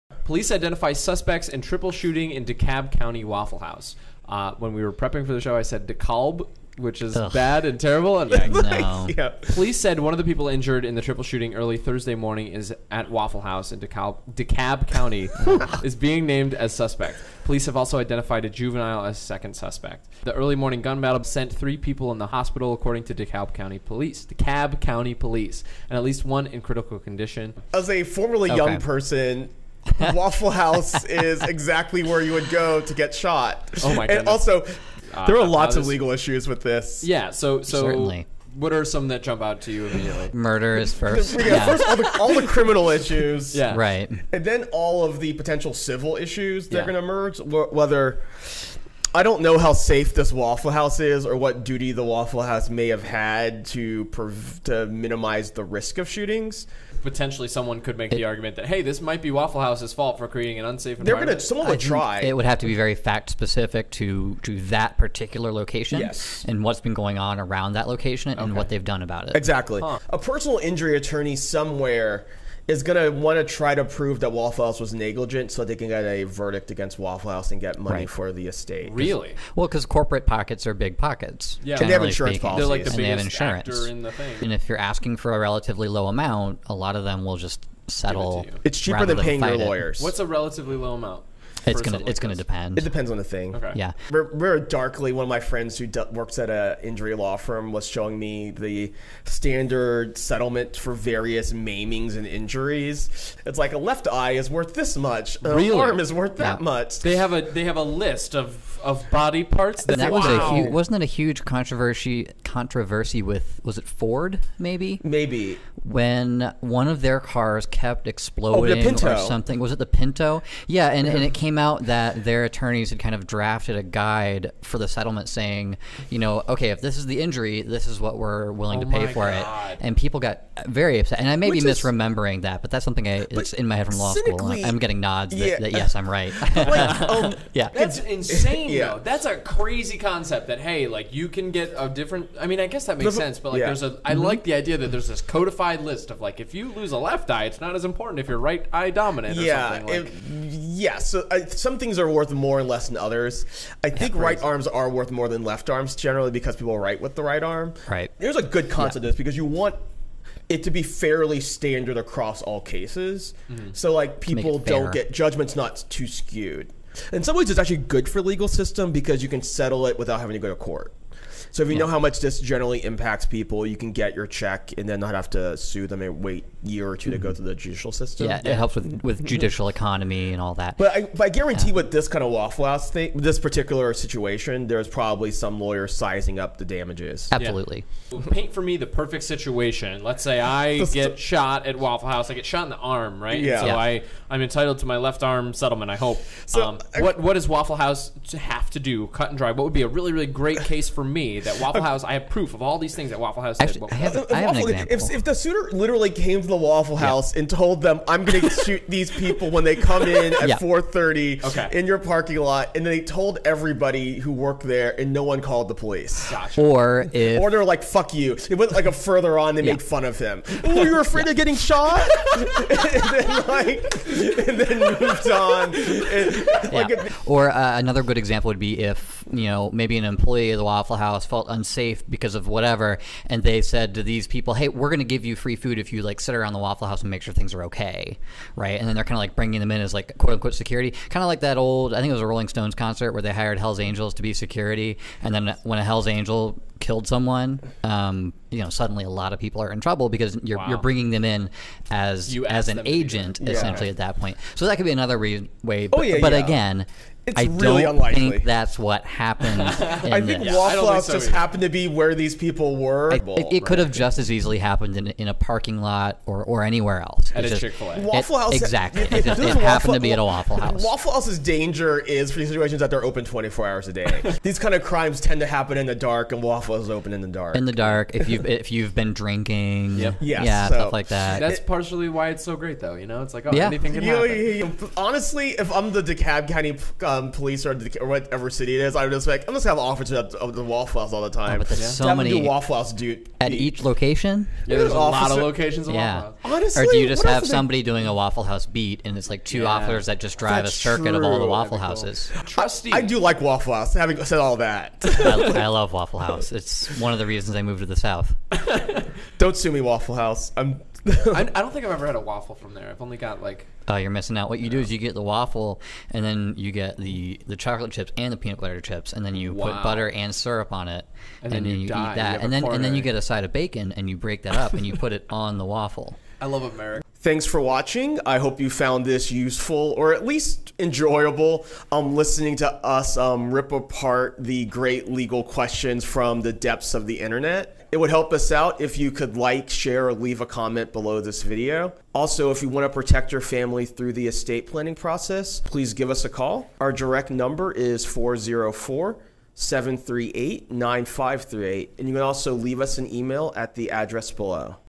Can you sue a restaurant if you get shot?
A triple shooting occurred late at night at a Waffle House in DeKalb County.

This raises the question of how safe these 24/7 restaurants are, and what they could do to prevent this from happening in the future.

And if you happen to be in this situation, can you sue the restaurant for the damages or injuries that you suffer?

Find out about it in today's episode!